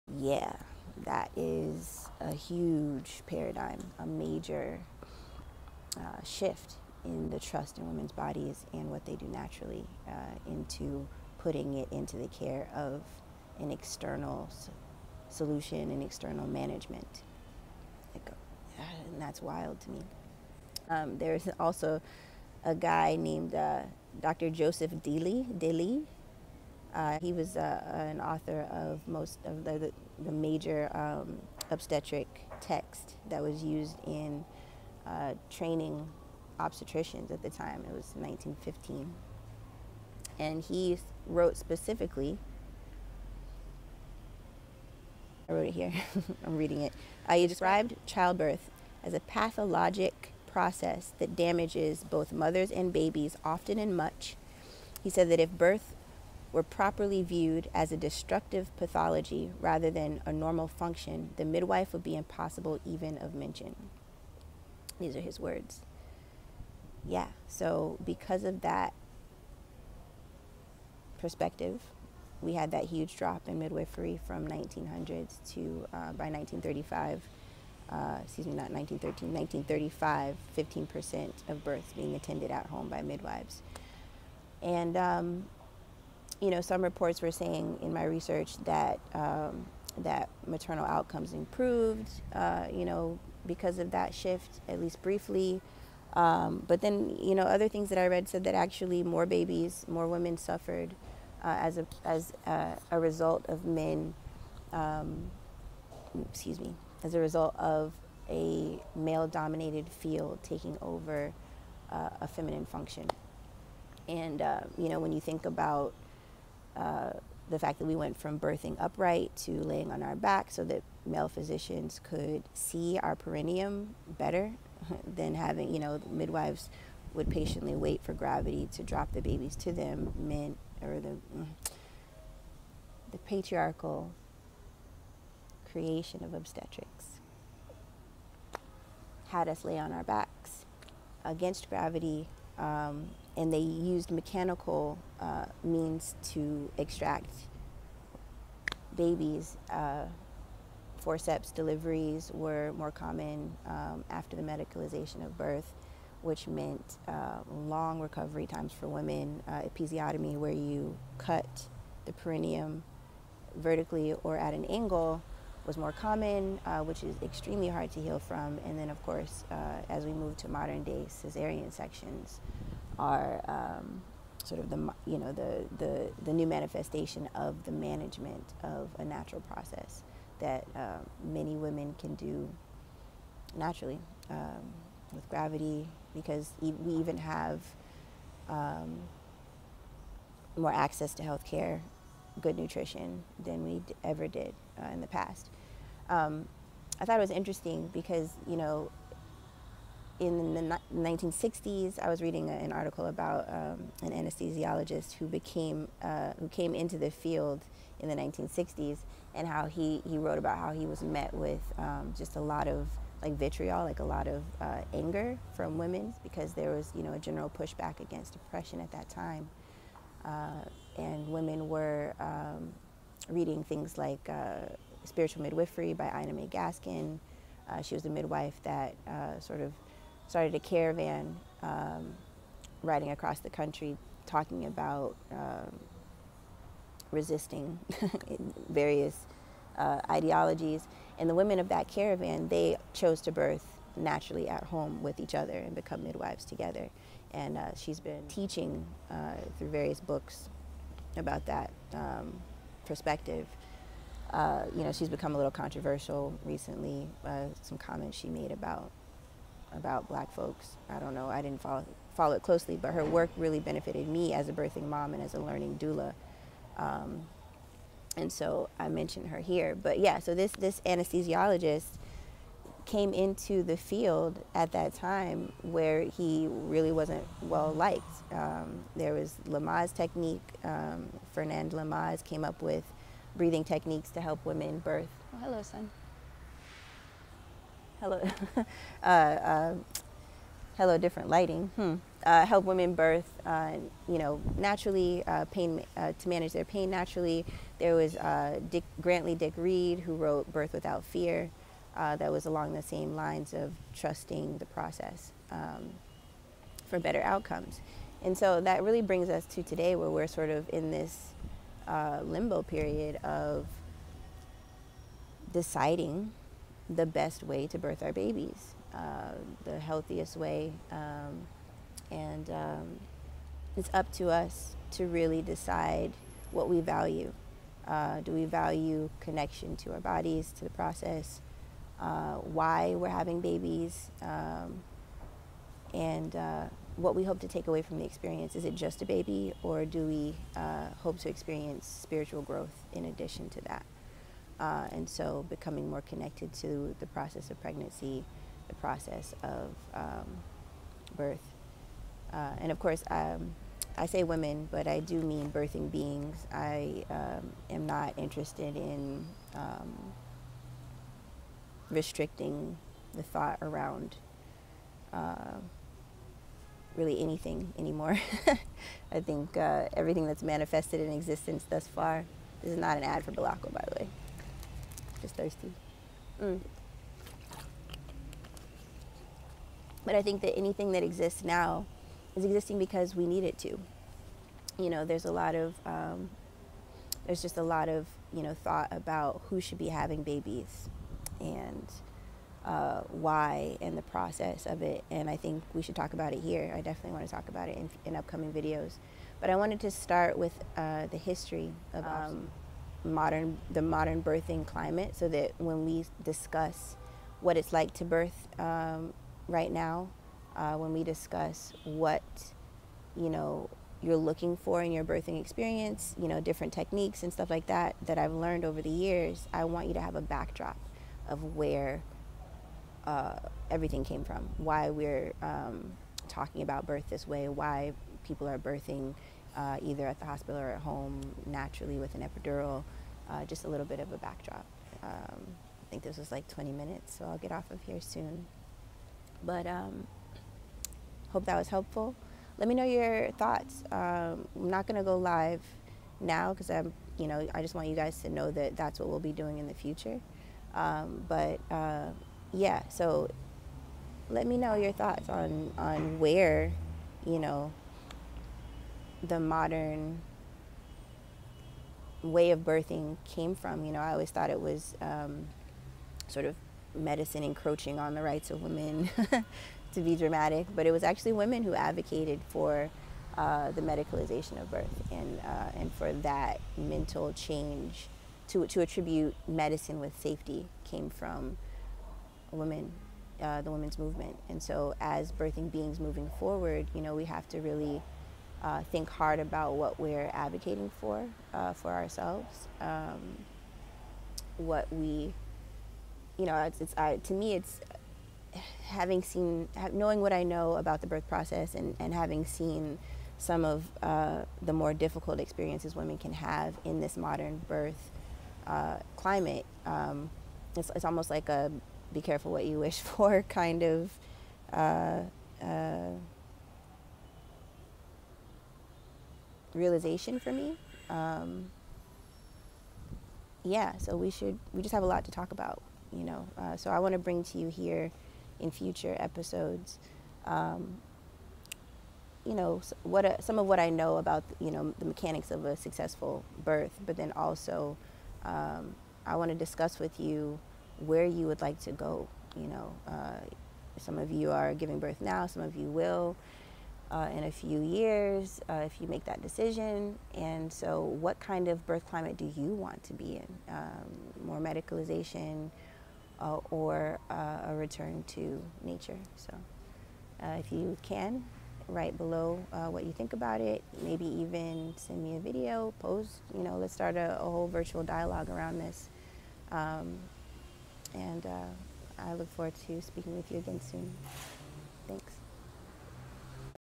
The ☀️ Rising ☀️ Doula Corner - Episode 1: A brief History of Obstetrics, Part 2
Weekly discussions with Zuri the Dreamer, a seasoned 🧂 🌶  Birth Doula. This is **PART 2 ** of the discussion, a brief history of Obstetrics in the West (other cultures have cooler histories/cultures, but we'll start with the one I earned my chops in).

Please excuse a couple of things: The rough editing - I didn't realize videos had to be under 15 mins for a newbie - and my rough appearance. Times are weird and it shows in my face. Get the message anyway 😆 The first half is in Part 1. Definitely go watch it *FIRST*.

I'm wanting this to be a place of discussion. Share your thoughts on the little I share here and in Part 1.  How does the switch in Birth Paradigms from Natural to Medical apply to your experience or your plans as a Life Creating Being? ♥️